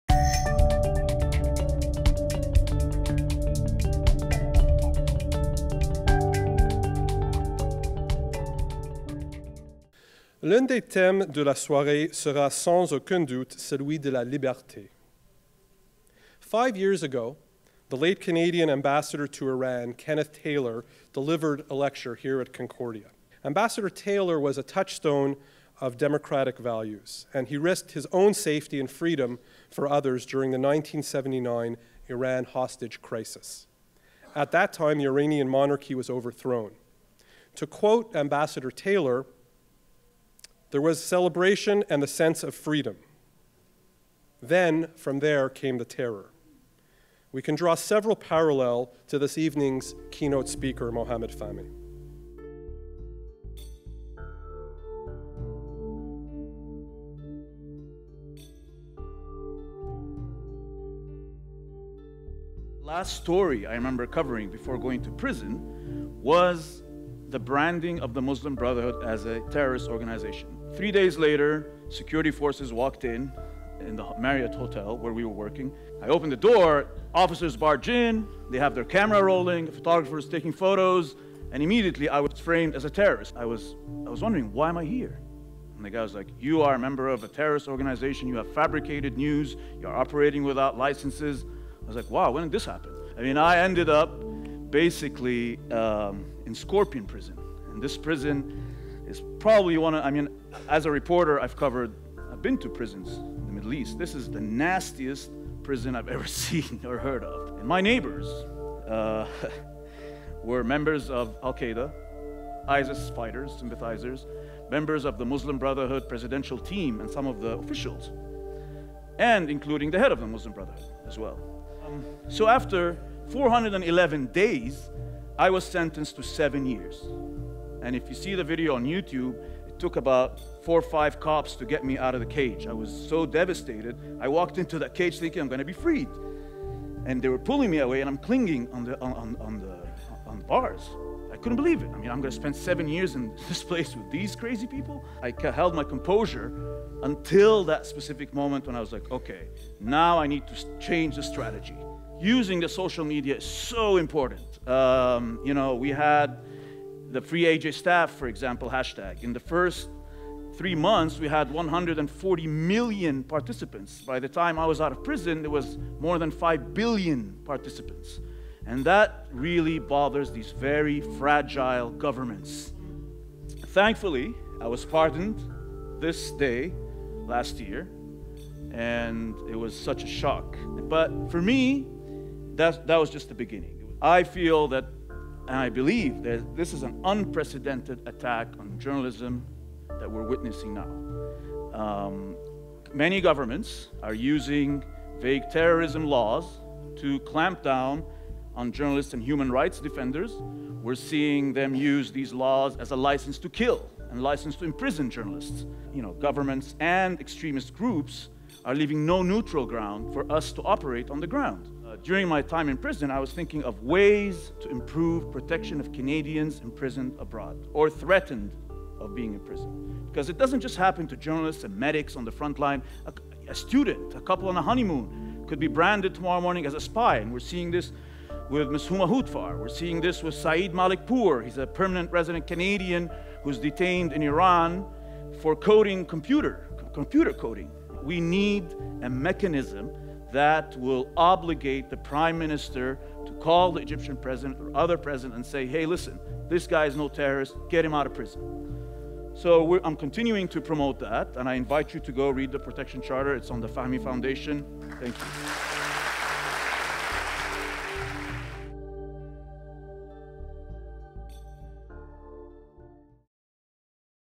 Mohamed Fahmy shares his incredible story at CUhomecoming
Mohamed Fahmy shares his incredible ordeal of arrest and imprisonment as a journalist covering the aftermath of Egypt's political uprisings.

After accepting the post of Al Jazeera English Bureau Chief in Cairo, Fahmy was falsely accused of being a member of the Muslim Brotherhood — a group designated a terrorist organization by the Egyptian government. He and his colleagues were imprisoned for more than 400 days alongside members of the Muslim Brotherhood, al-Qaeda and ISIS.

Following massive international outcry, Fahmy was finally pardoned of all charges in September 2015.